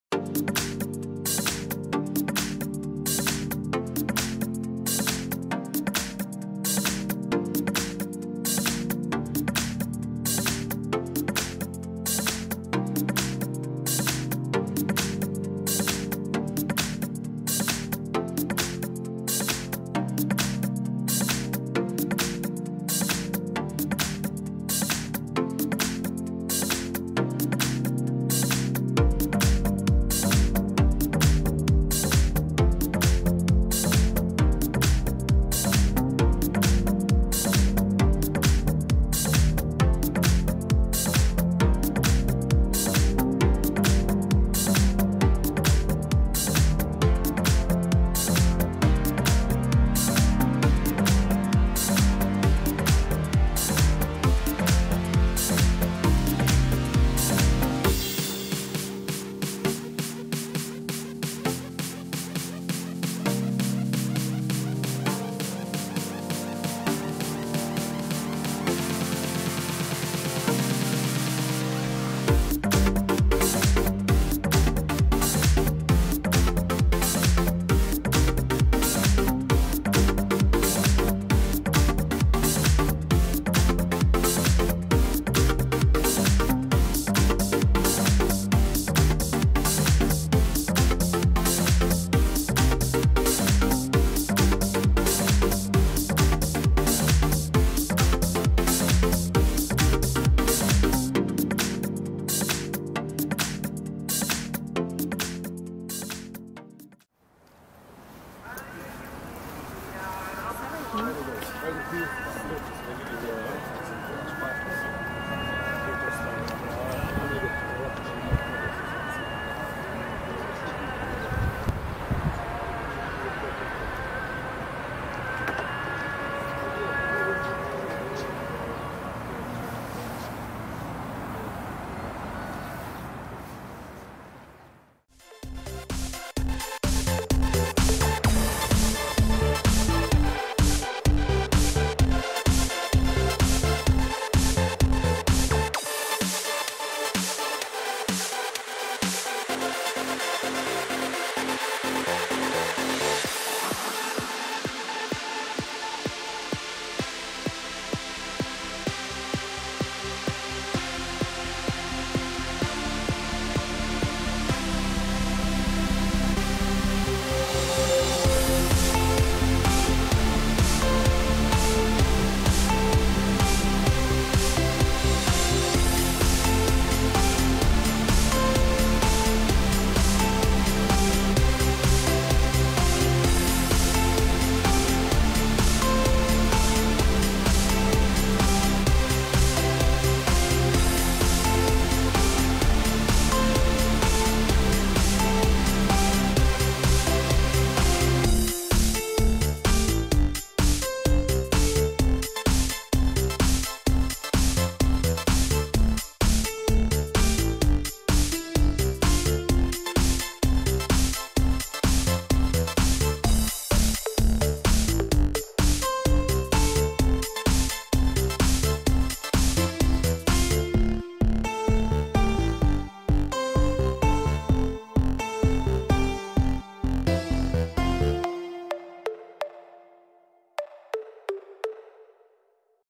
Ear Popping Ascend to Avaz Twist Tower
Lift 35 floors in seconds to see the majestic view of Sarajevo